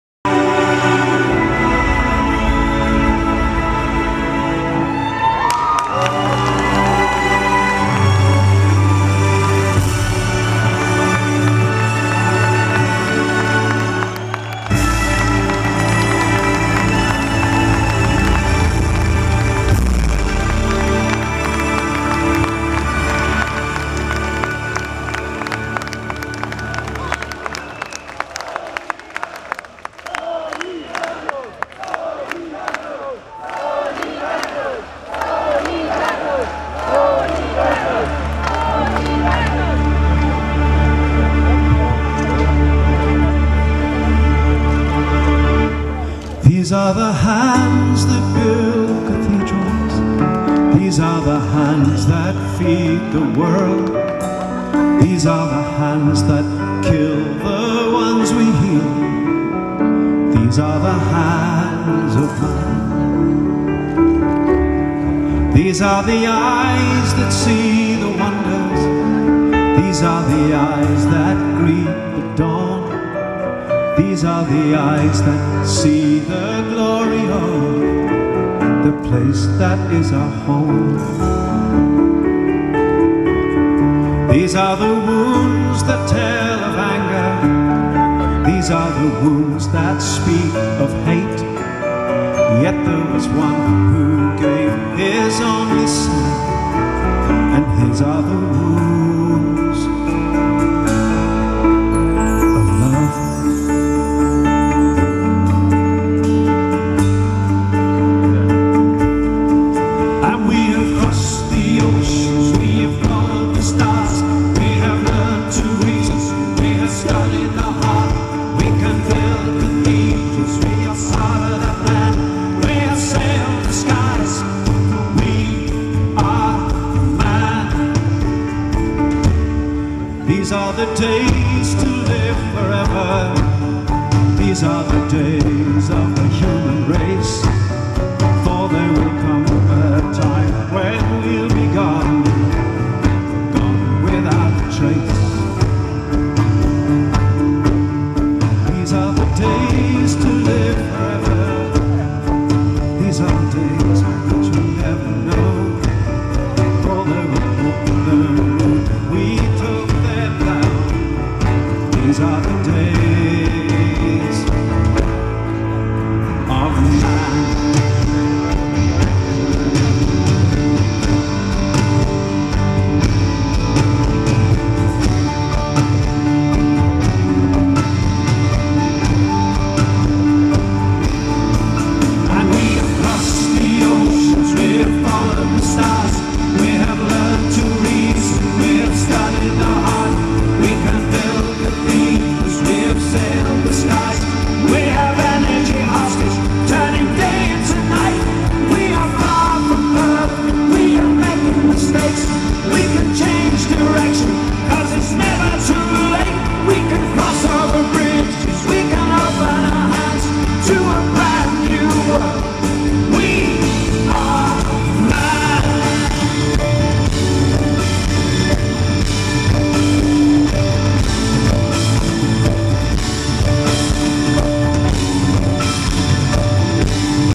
Chris de Burgh -Solidarność Pokoleń-Gdańsk-Prolog Koncertu.
Wzruszający początek koncertu który odbył się na terenach Stoczni Gdańskiej w sobotę 29 sierpnia 2015 roku z okazji 35 rocznicy powstania Solidarności.
Andrzej Adamczyk
To jest Mielec
Stowarzyszenie Orzeł Biały- Strzelec